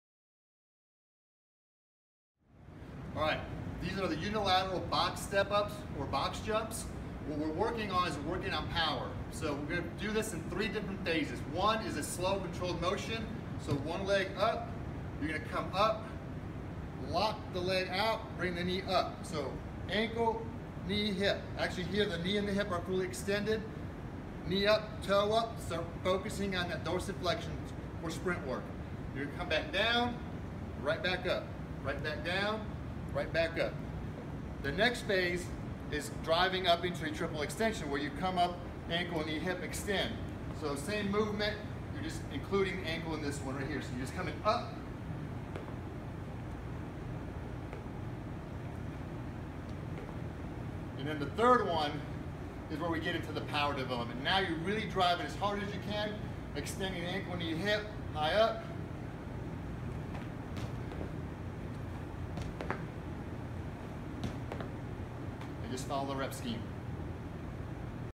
Unilateral Box Step Ups
Instructional video on how to perform Unilateral Box Step Ups